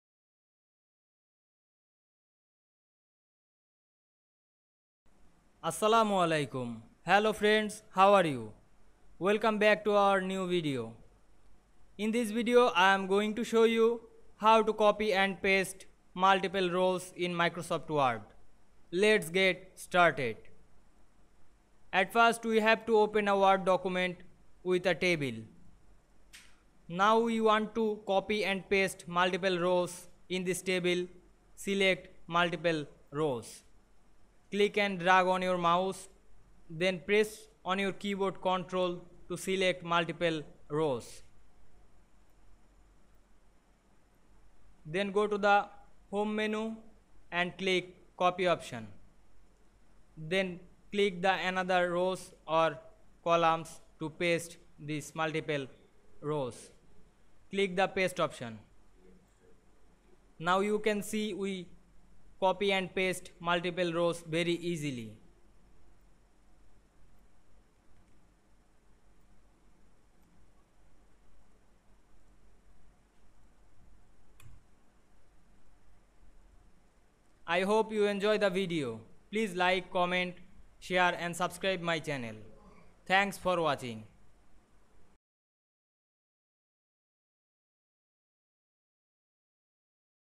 How to copy and paste multiple rows in word
In this video I will show you: How to copy and paste multiple rows in word